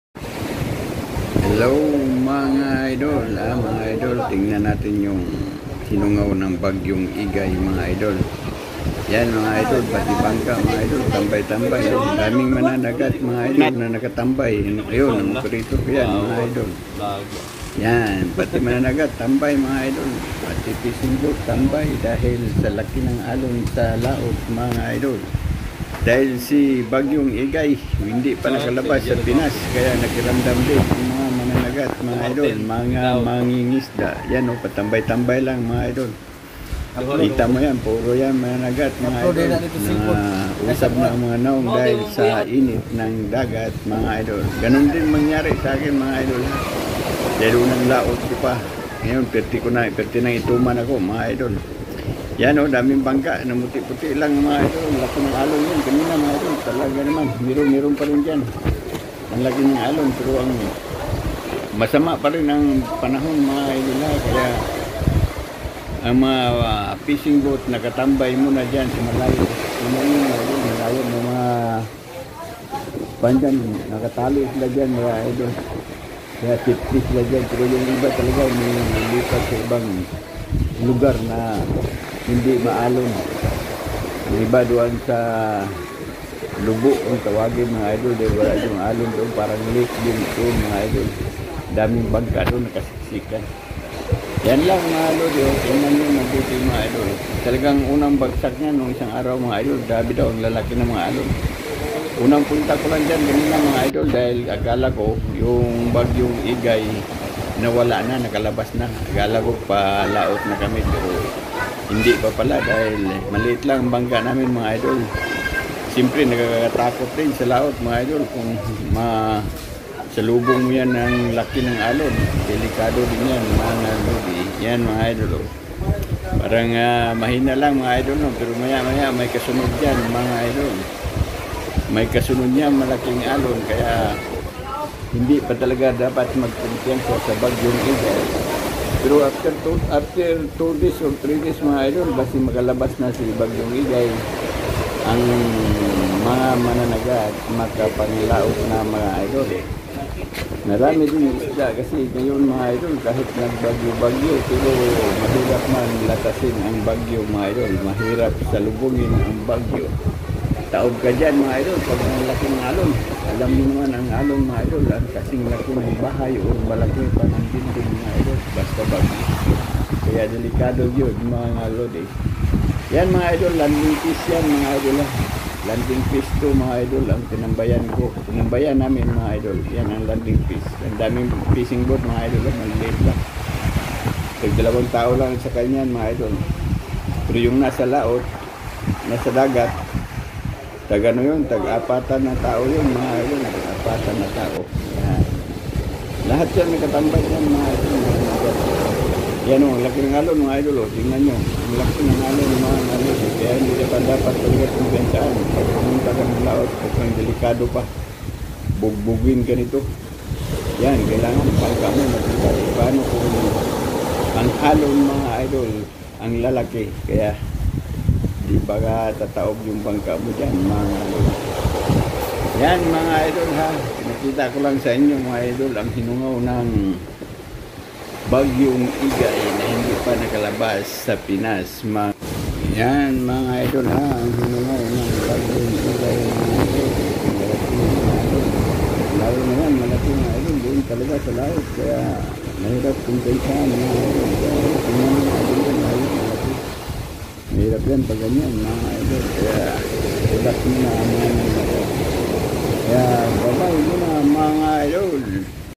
nasa pinas pa si bagyong egay kaya tambay muna ang mga mangingisda @ badz-mix's vlog
hinungaw ni bagyong egay @ badz-mix's vlog